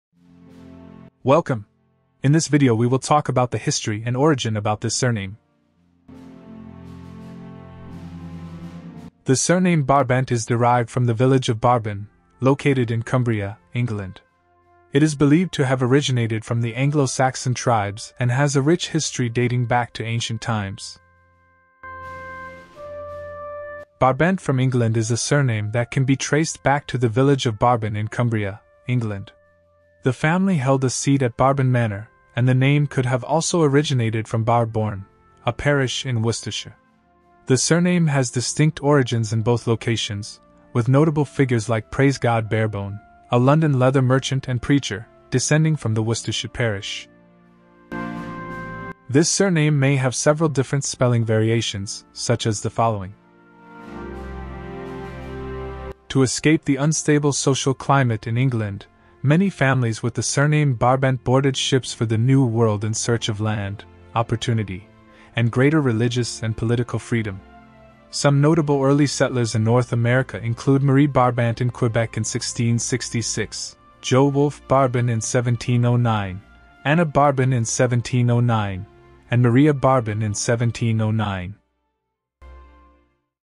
Barbent from England 🏴󠁧󠁢󠁥󠁮󠁧󠁿 - Meaning, Origin, History & Migration Routes of Surname
The surname Barbent is derived from the village of Barbon, located in Cumbria, England. It is believed to have originated from the Anglo-Saxon tribes and has a rich history dating back to ancient times.

Barbent from England is a surname that can be traced back to the village of Barbon in Cumbria, England. The family held a seat at Barbon Manor and the name could have also originated from Barbourne, a parish in Worcestershire. The surname has distinct origins in both locations, with notable figures like Praise-God Barebone, a London leather merchant and preacher, descending from the Worcestershire parish.